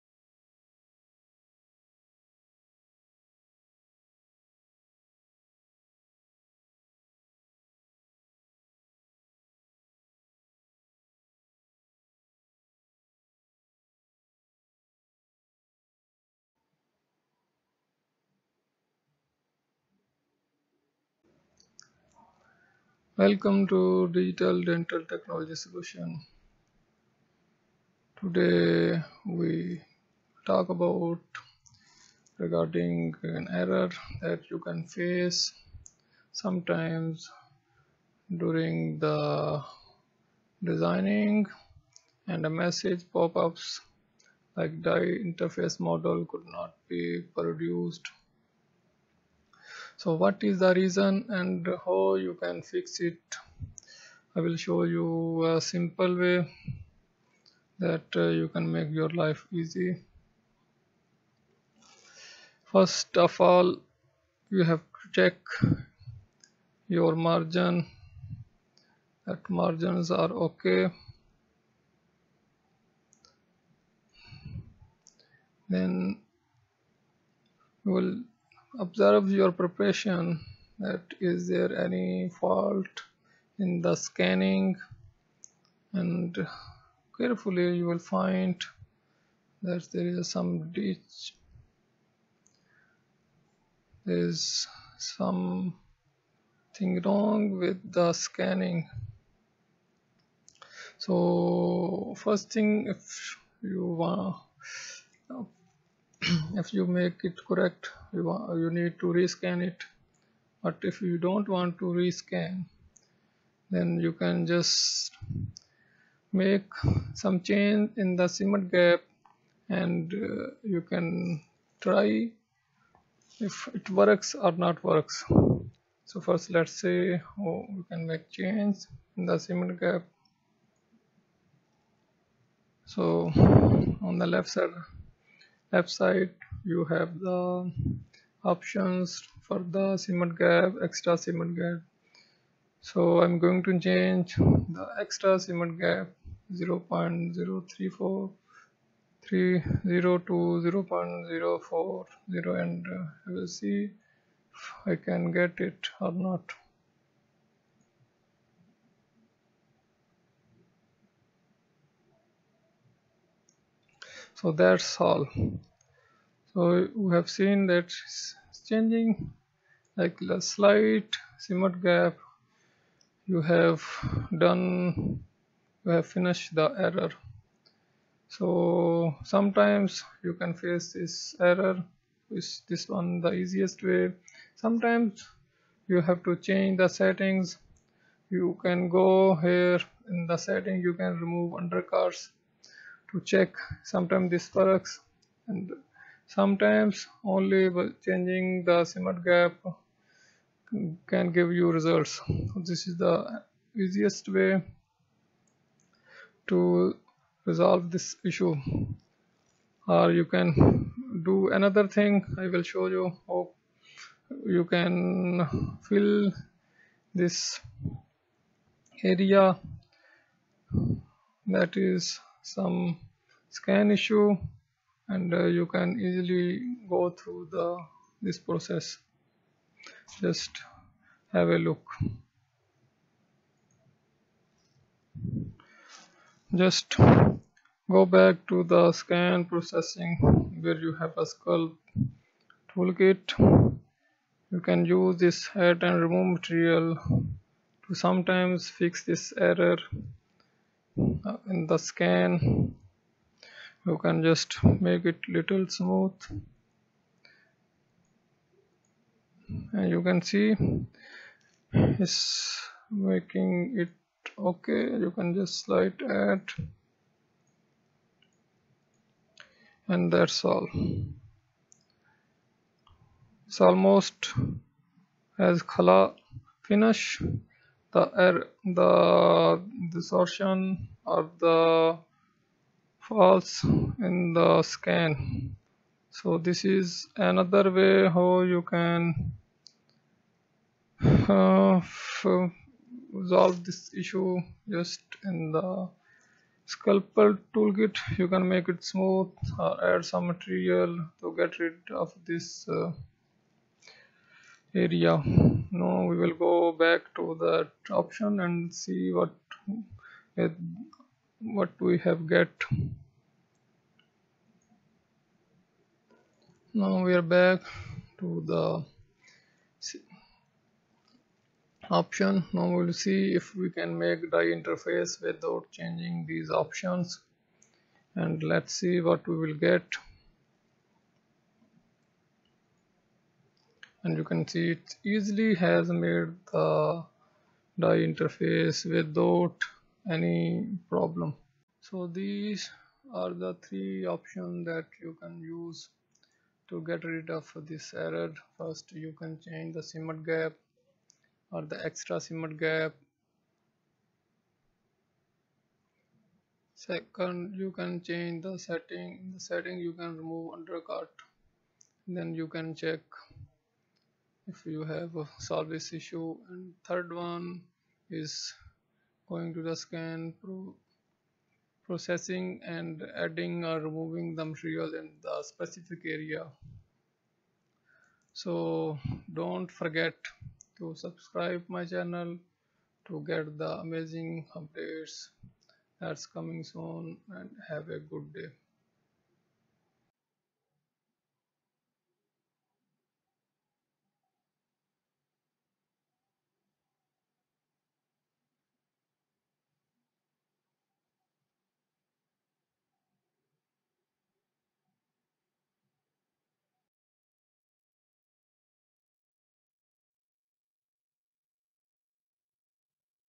die interface error how to fix it in 3shape dental designer
This is a DDTS production for 3Shape users. This Video is a part of amazing solutions for 3shape users.This part will show you how you can fix an error ,die interface model could not be produced, Using simple tips and tricks that i have learned over the years using the software.